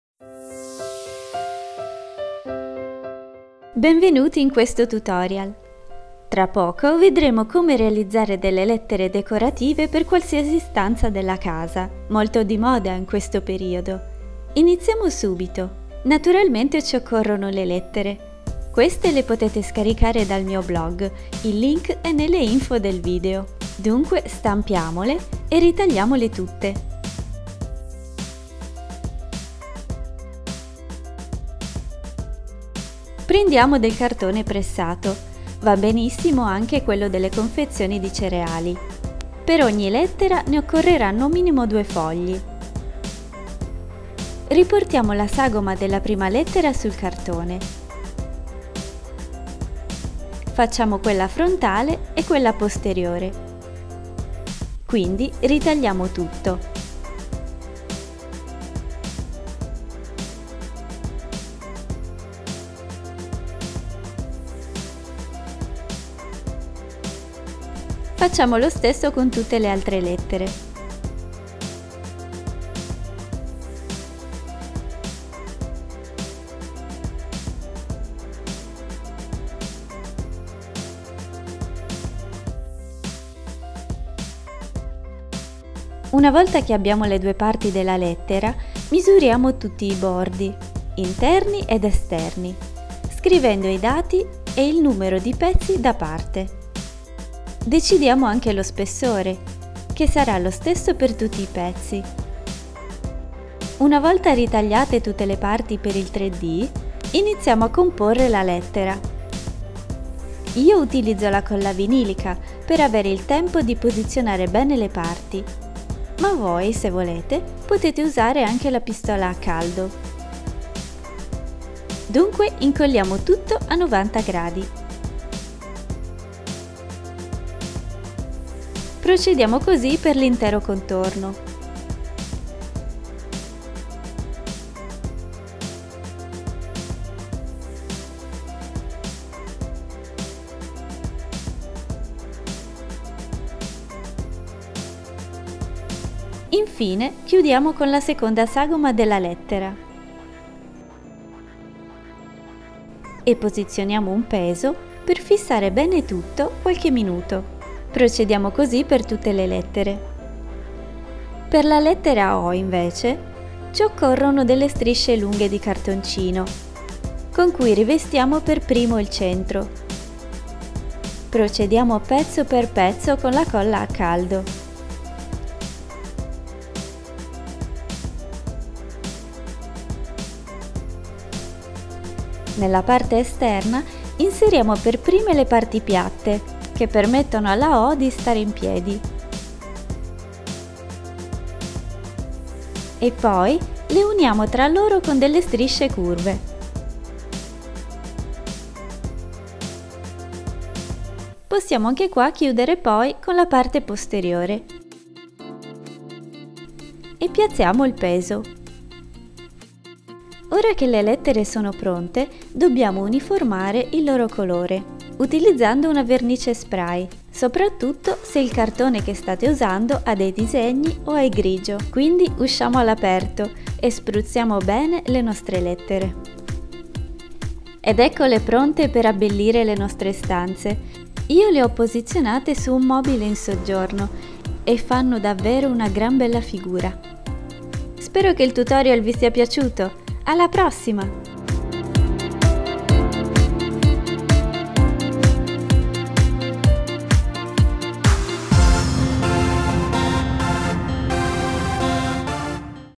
Lettere in 3D MODELLO GRATUITO - Home decor 3D Letters DIY
lettere 3D per decorare la casa, buona visione!

Cosa ho utilizzato:
– Cartone pressato
– Tappetino da taglio
– Colla vinilica
– Colla a caldo
– Vernice acrilica spray

SweetBioDesign